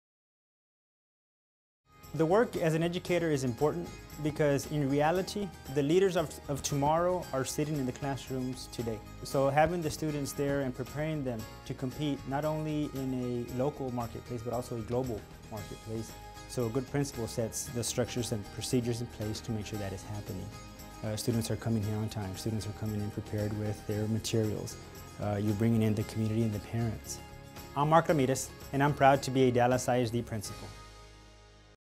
Meet Mark Ramirez - Principal, Molina High School
Ramirez discusses what makes an effective Principal.